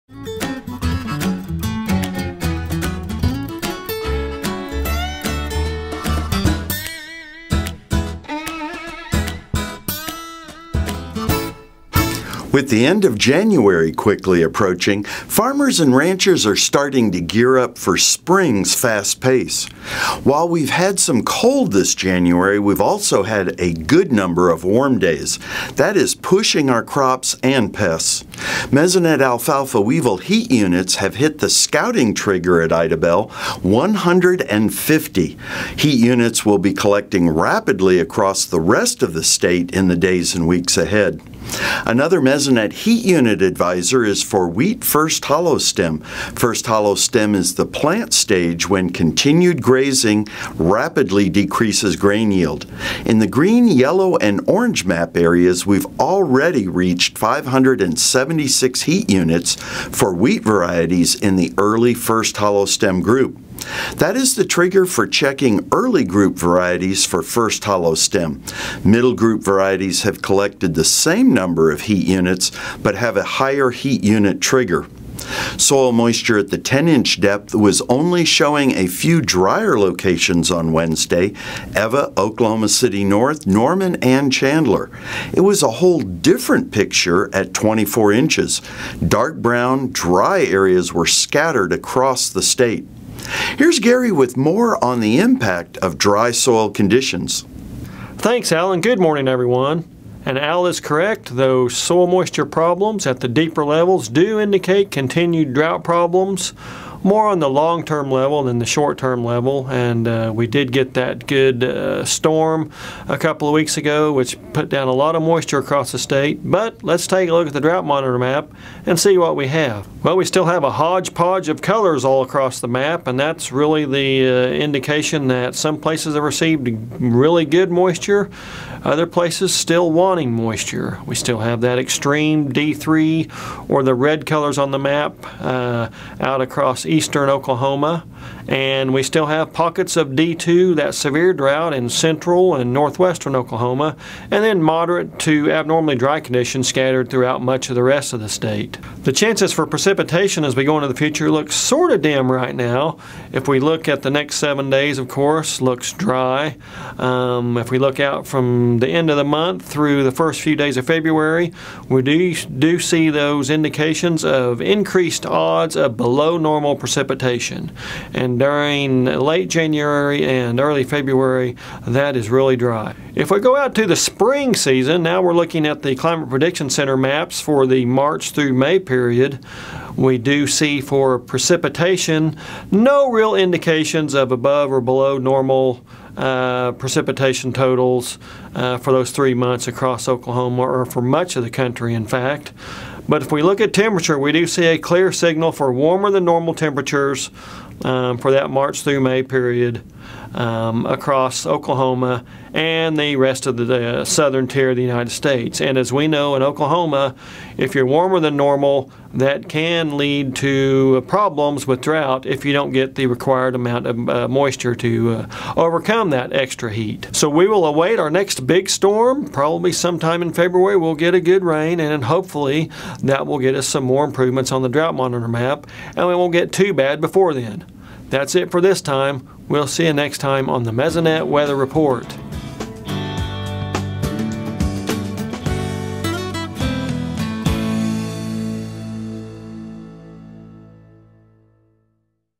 Mesonet Weather (1/28/17)
Al Sutherland shows the areas of the state that should be scouting for first hollow stem and weevils. Gary McManus has a dry and warm 90-day outlook.